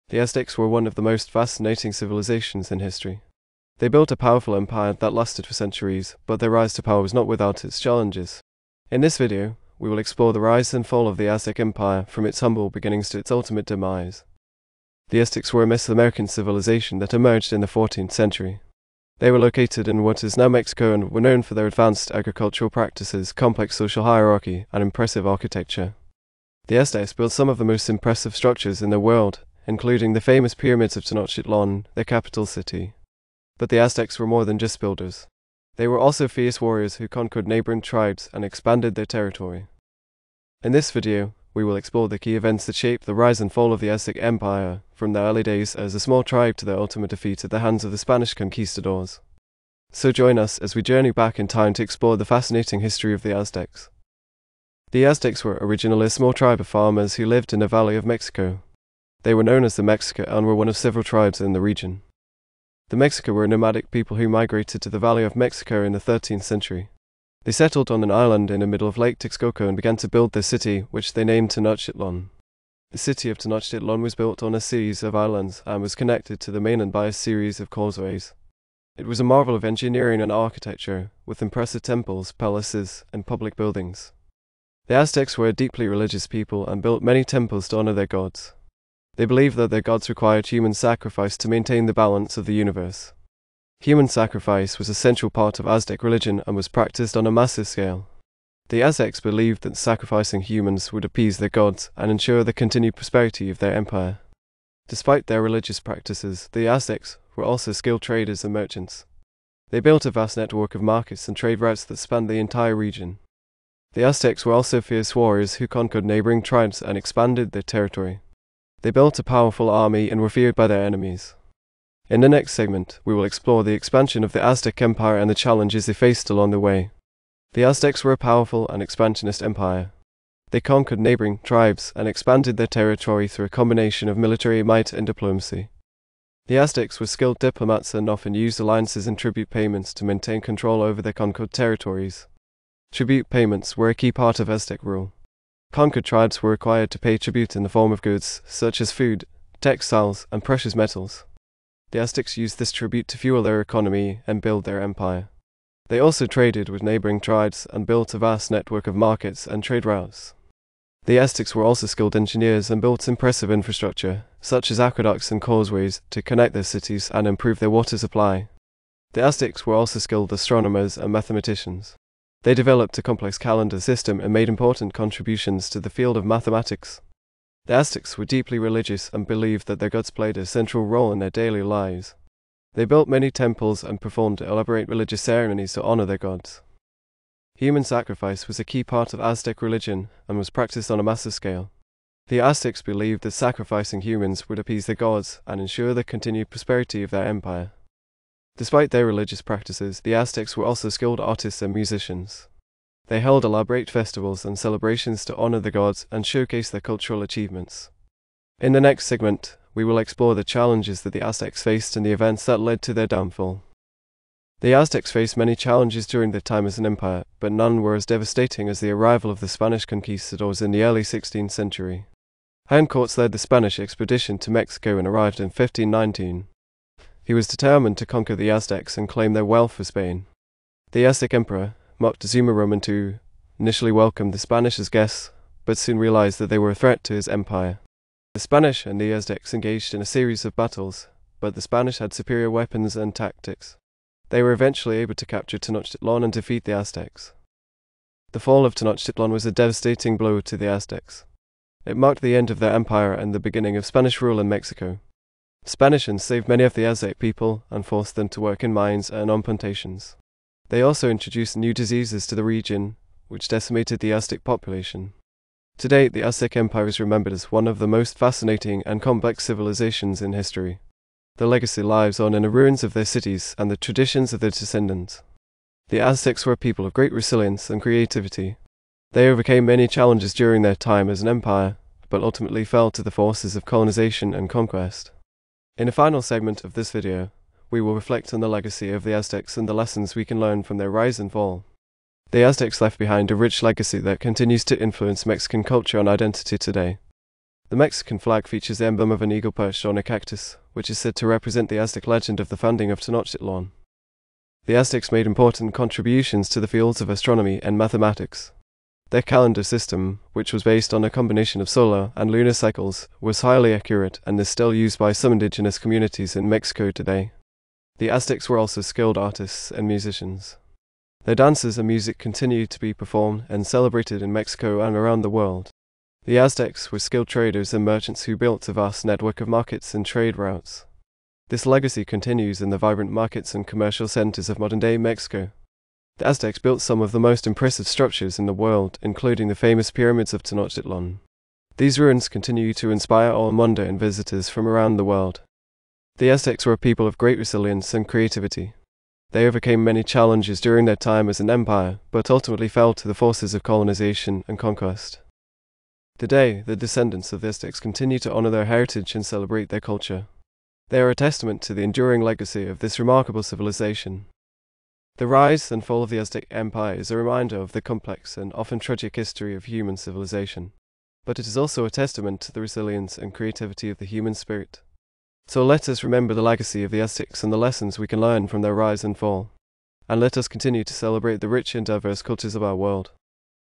The Aztecs: The Rise and Fall of an Empire
In this fascinating video, we delve into the history of the Aztecs, one of the most powerful empires in the world. From their humble beginnings to their eventual downfall, we explore the key events and figures that shaped their rise and fall. With stunning visuals and expert analysis, this video offers a comprehensive look at the Aztec civilization and its enduring legacy. Whether you're a history buff or simply curious about this fascinating culture, this video is a must-watch. So sit back, relax, and join us on a journey through the rise and fall of the Aztec Empire.